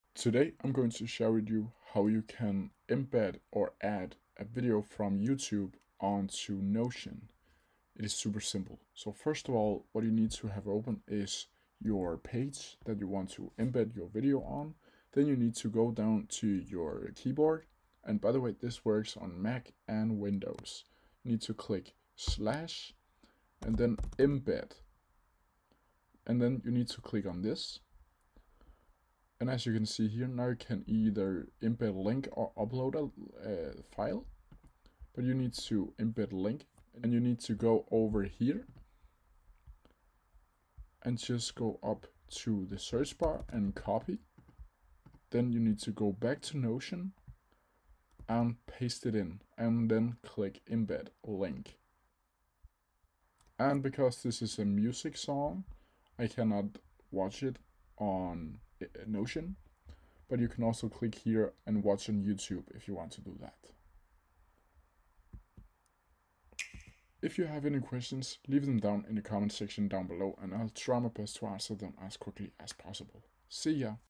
How To Embed Videos on Notion
In this Notion tutorial i'll be showing you How To Embed Videos on Notion. You can embed videos from anywhere including YouTube, Vimeo, Rumble and more.

In this specific Notion Tutorial i'm showing you how to embed / add a youtube video unto Notion.